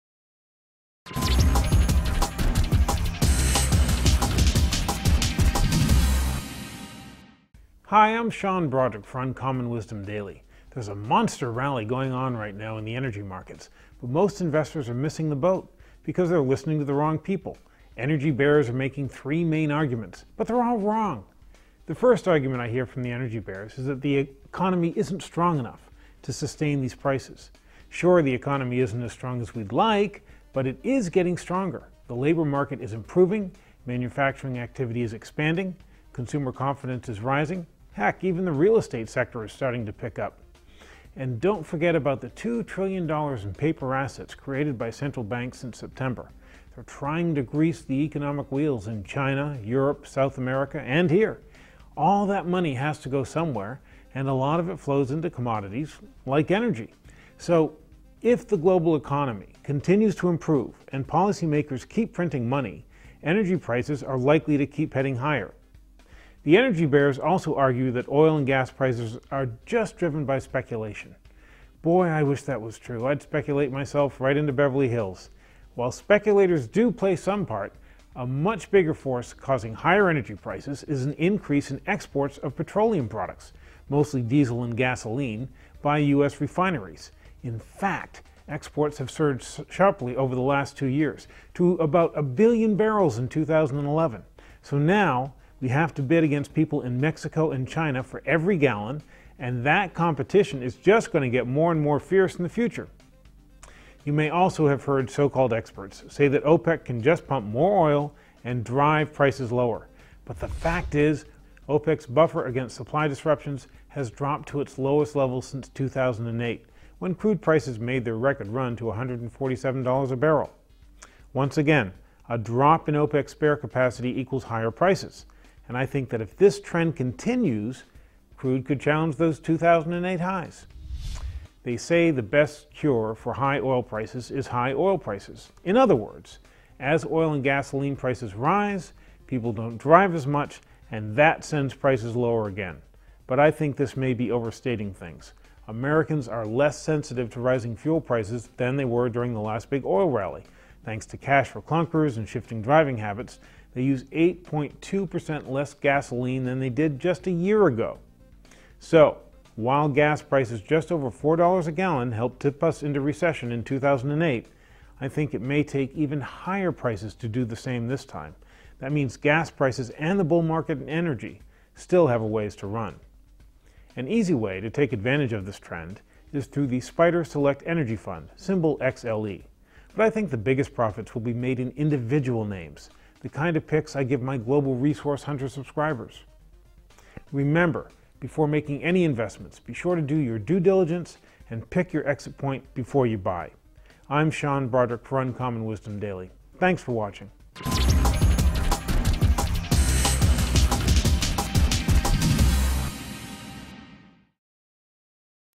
Why You're Missing the Big Rally in Energy
Energy bears argue that the bull market in oil and gas futures is over. But Sean Brodrick thinks it still has a ways to run, due to an improving economy and money printing by central banks, increased global demand and a lack of spare supply.